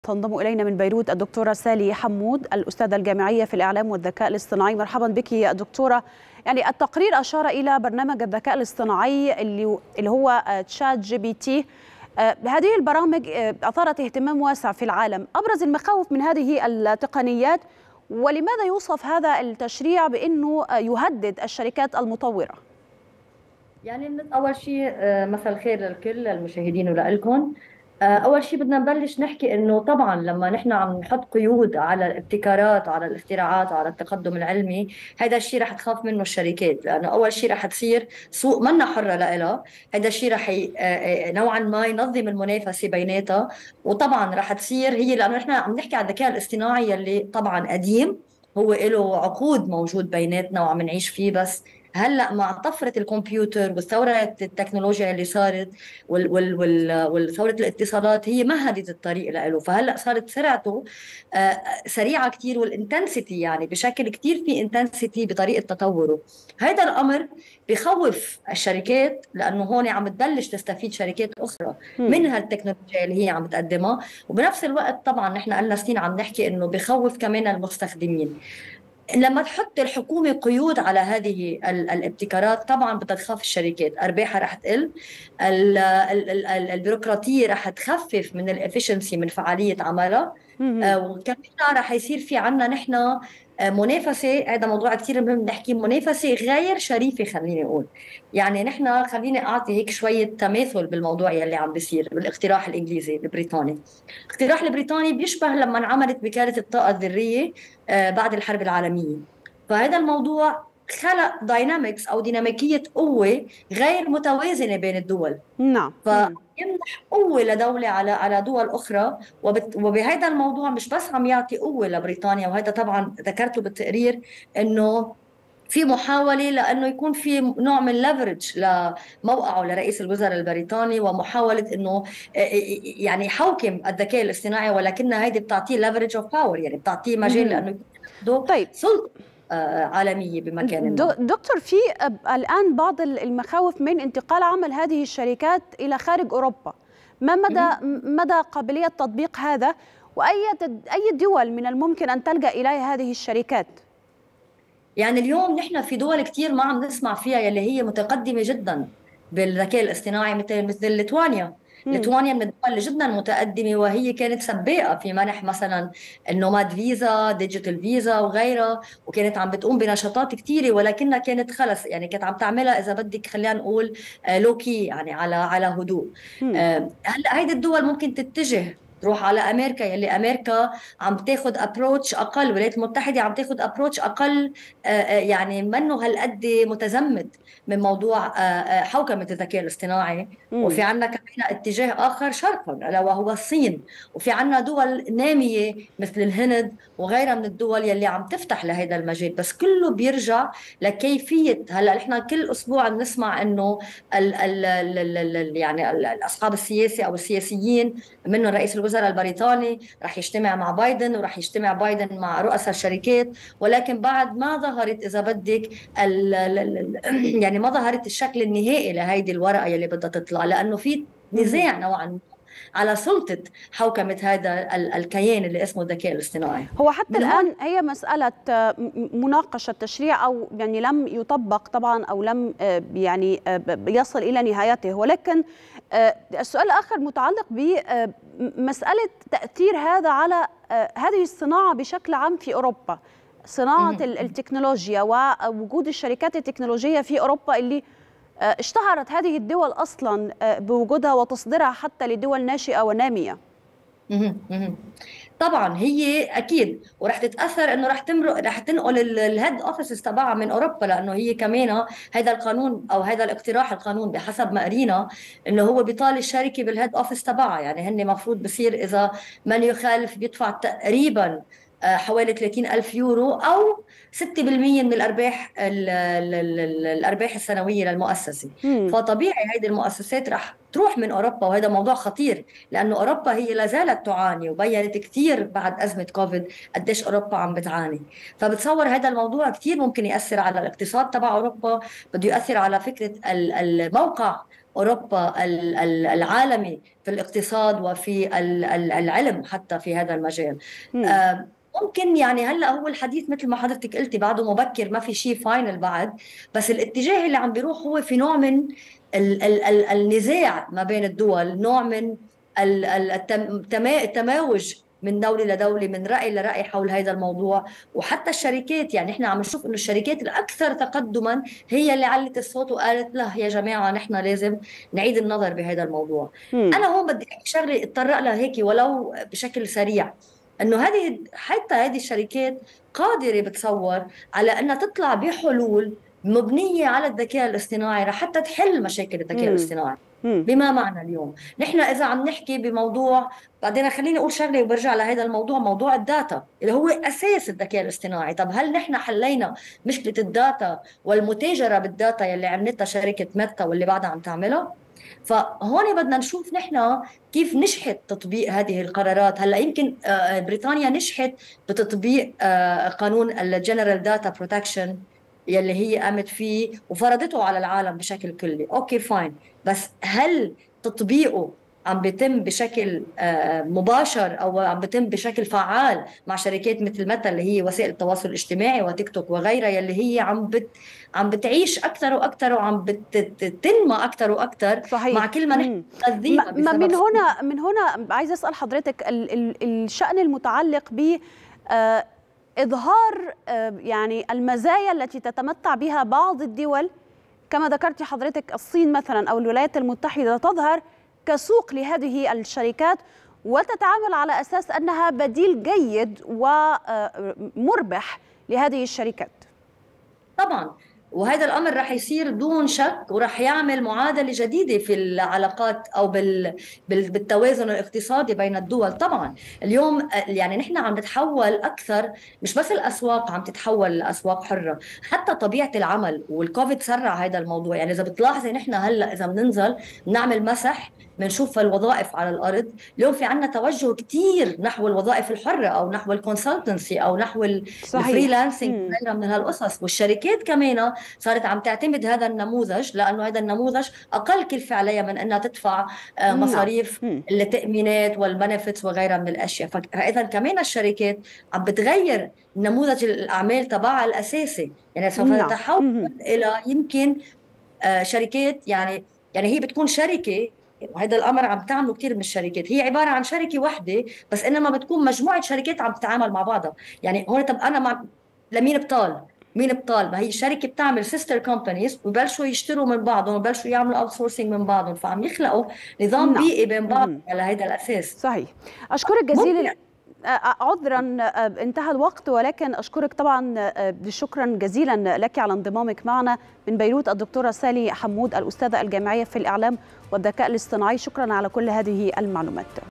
د.سالي حمود أستاذة الذكاء الاصطناعي:التكنولوجيا الجديدة خلقت منافسة غير شريفة وتهديد كبير على أوروبا
30/6/2023